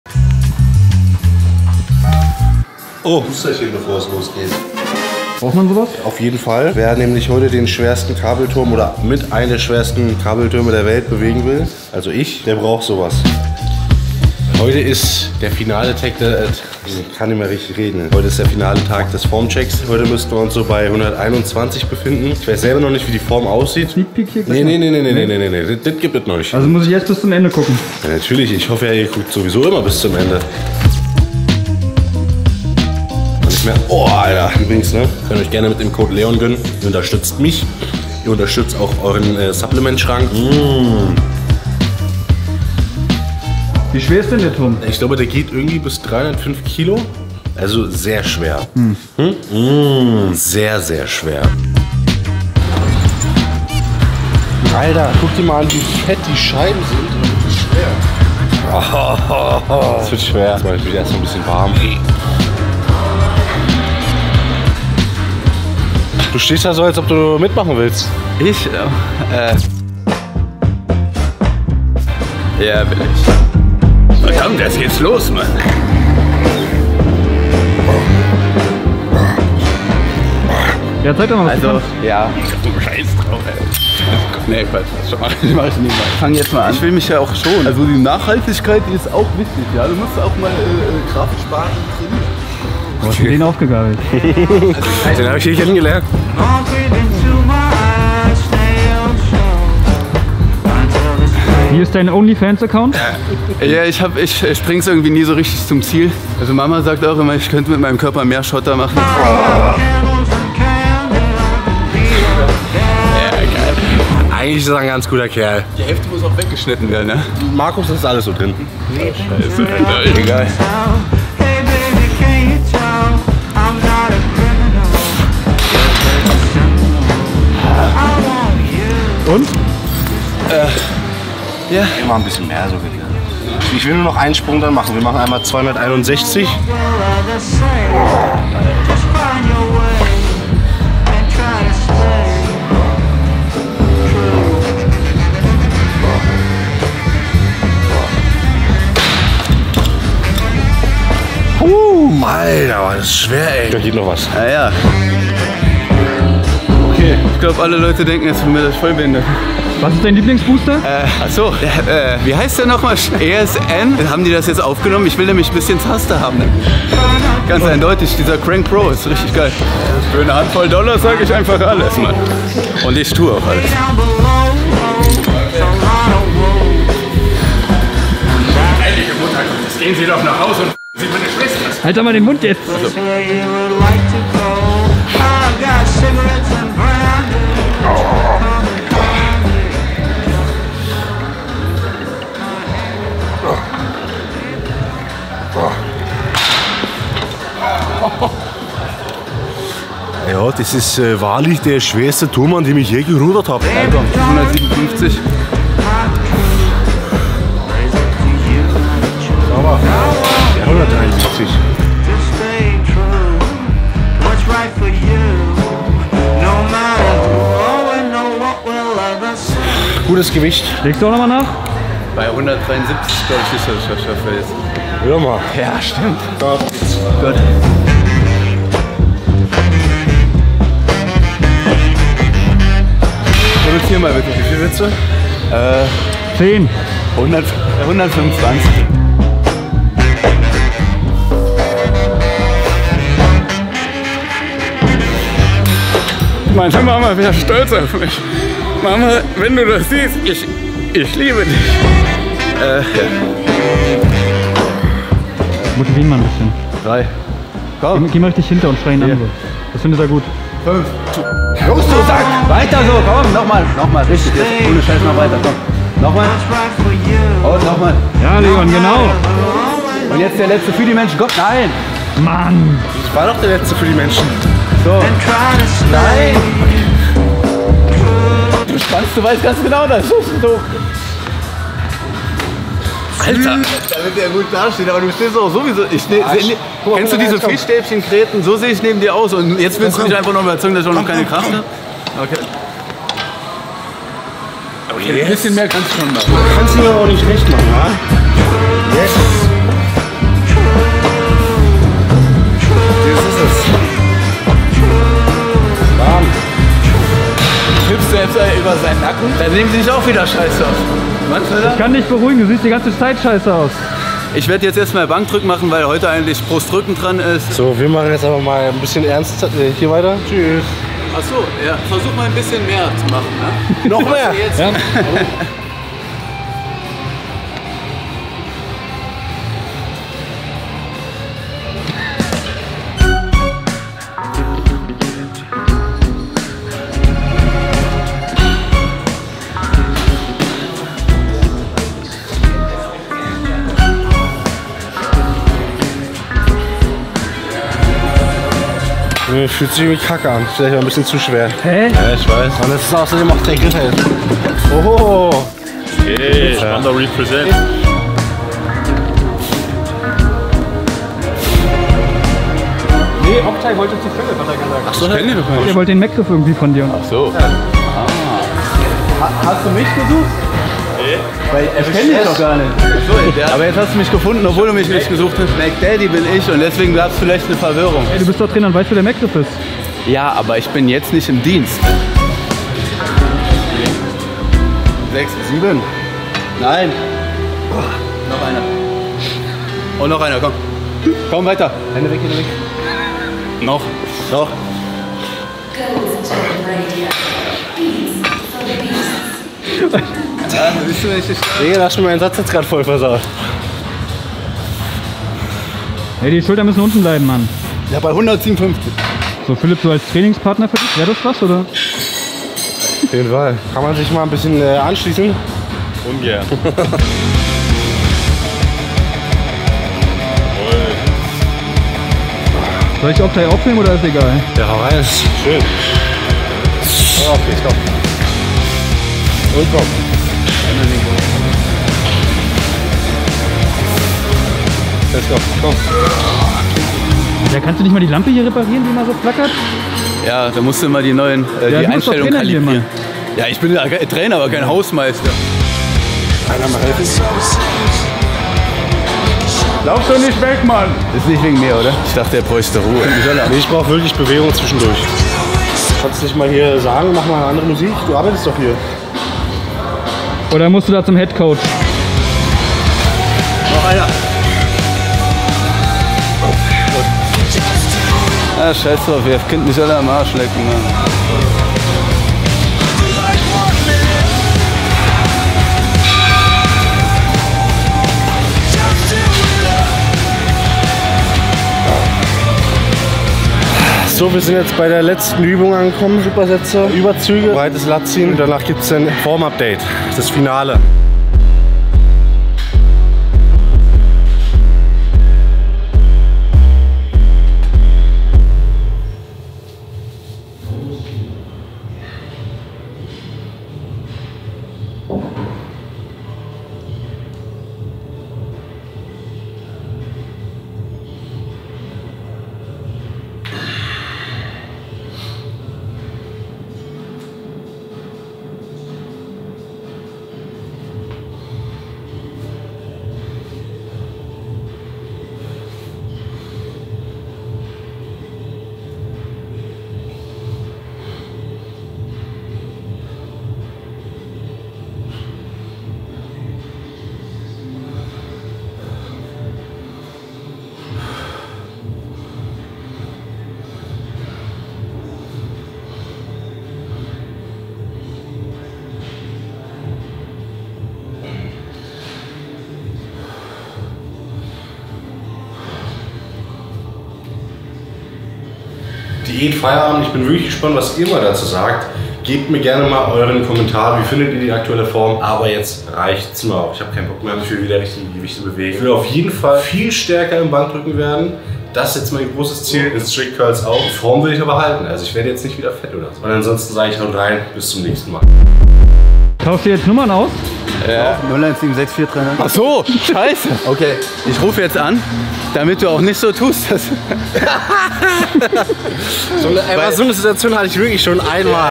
309kg Rudern am Kabel
Wie immer dachte ich mir: "Komm, ruder mal den ganzen Stack." Blöd nur, dass es dieses mal bis 309kg ging. Angeblich auch einer der schwersten Türme der Welt. Genau was für mich. Naja Ergebnis seht ihr im Video. Außerdem gibts n kleines Fazit zu meiner Form, da ich jetzt nicht weiter Diäten werde (vorerst), damit ich wieder stärker beim Bankdrücken & Strict-Curlen werden kann (aktuelles Ziel). Meine Form will ich trotzdem halten, also fett werden ist erstmal nicht drin und mMn auch nicht notwendig.

*Werbung